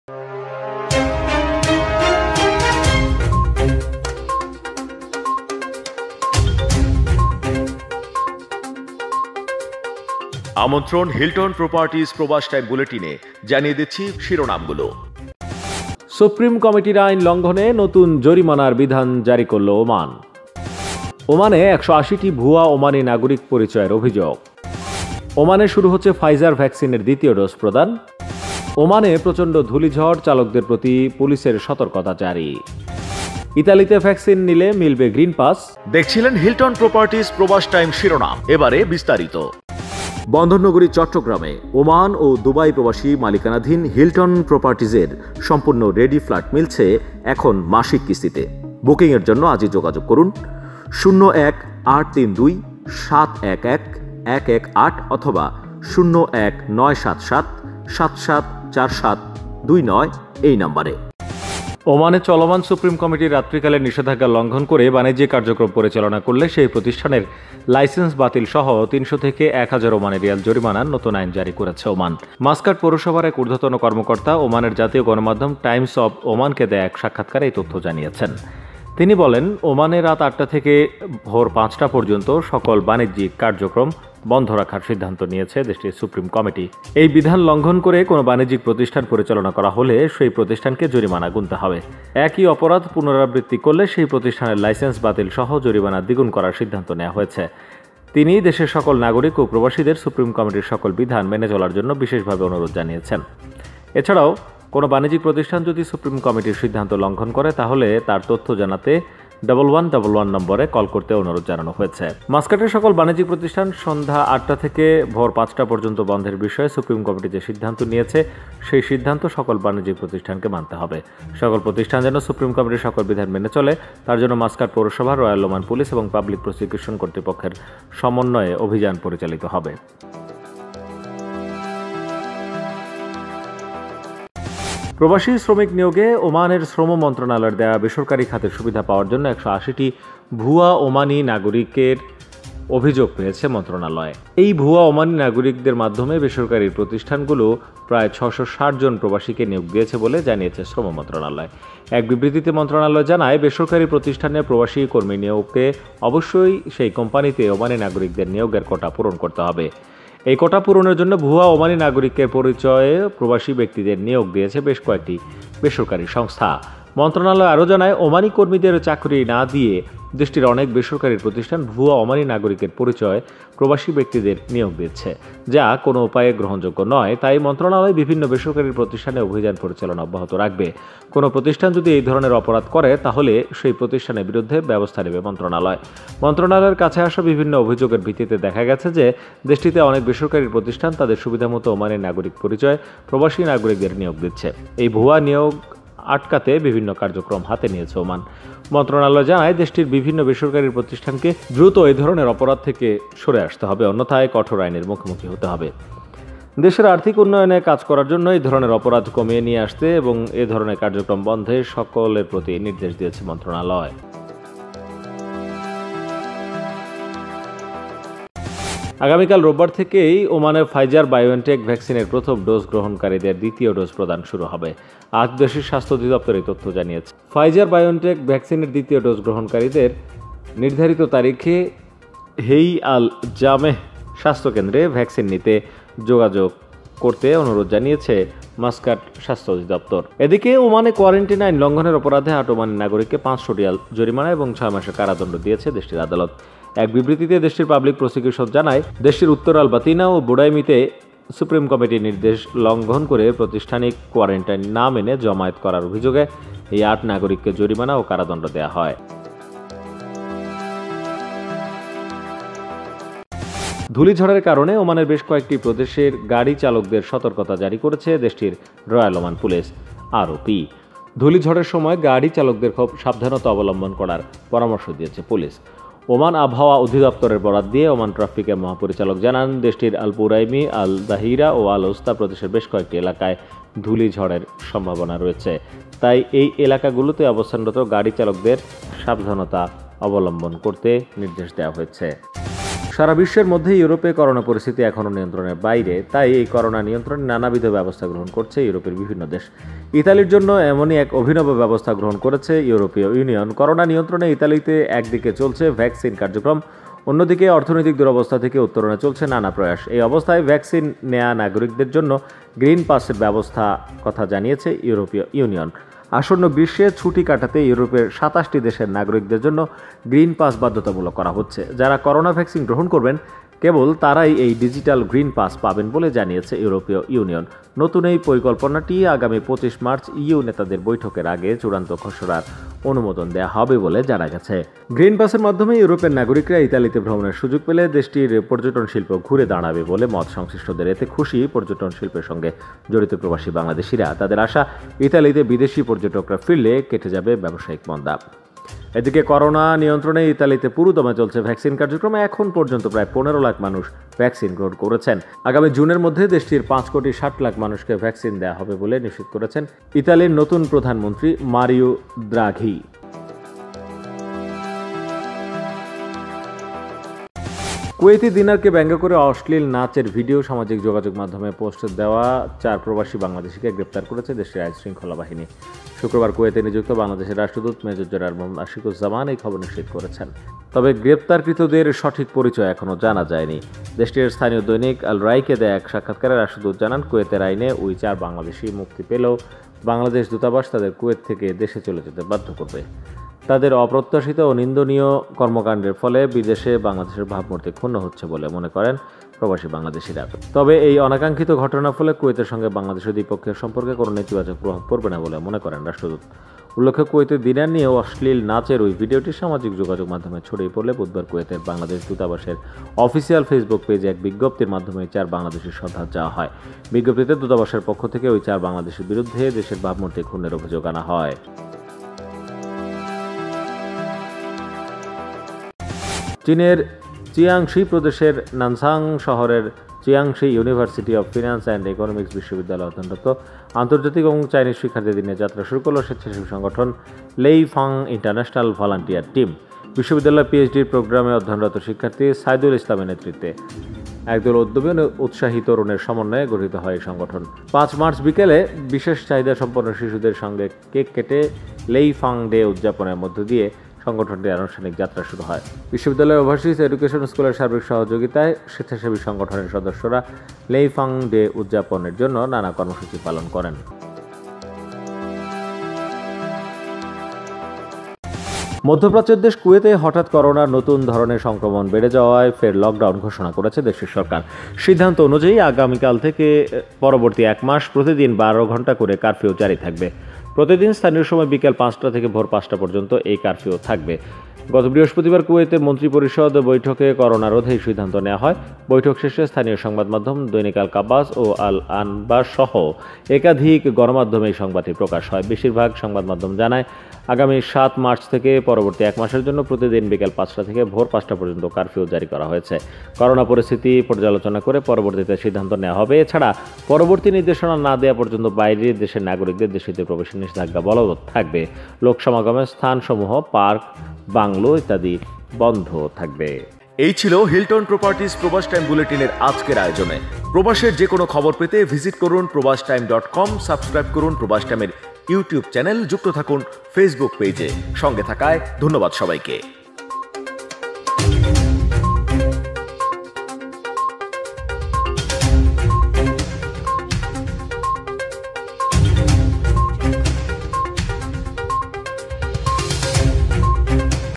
ওমানের আজকের গুরুত্বপূর্ণ সংবাদ
শিরোনাম
১-সুপ্রিম কমিটির আইন লঙ্ঘনে নতুন জরিমানার বিধান জারি করলো ওমান
২- ওমানে ১৮০ টি ভূয়া ওমানি নাগরিক পরিচয়ের অভিযোগ
৩- ওমানে শুরু হচ্ছে ফাইজার ভ্যাকসিনের দ্বিতীয় ডোজ প্রদান
৪- ওমানে প্রচন্ড ধূলিঝড়, চালকদের প্রতি পুলিশের সতর্কতা জারি
৫- ইতালিতে ভ্যাকসিন নিলে মিলবে গ্রিন পাস
৬- কুয়েতে ‘অশ্লীল নাচ’ করা সেই ৪ বাংলাদেশি প্রবাসী গ্রেফতার
৭- চীনে বাংলাদেশি তরুণের নেতৃত্বে মানবিক সংগঠনের যাত্রা
৮- মহামারী করোনার প্রকোপে কুয়েতে ফের কারফিউ জারি

- Team Probash